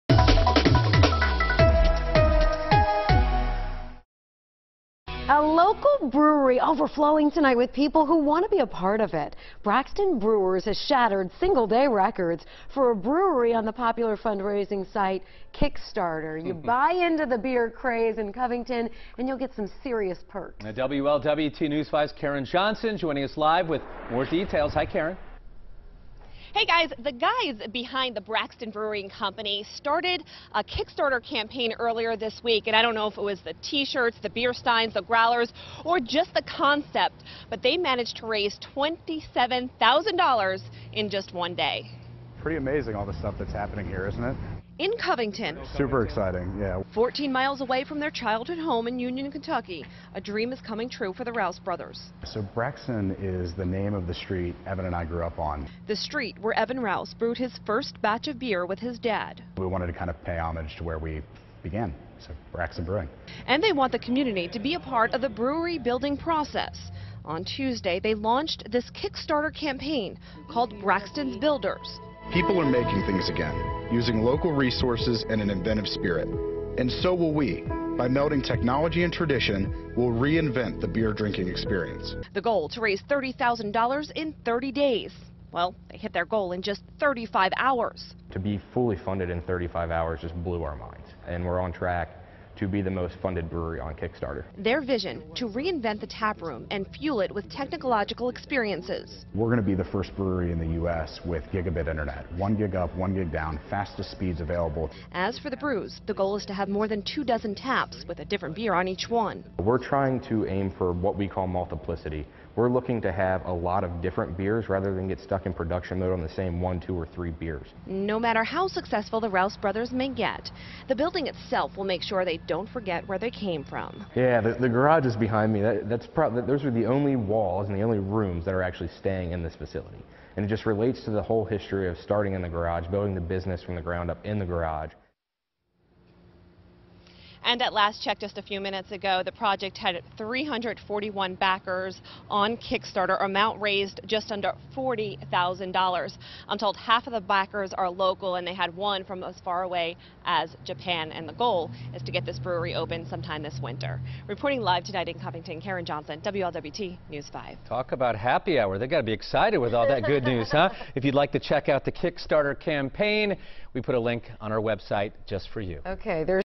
Brothers planning Covington brewery break single-day Kickstarter records
The Braxton Brewing Company has shattered single day records for a brewery on the popular fundraising site Kickstarter.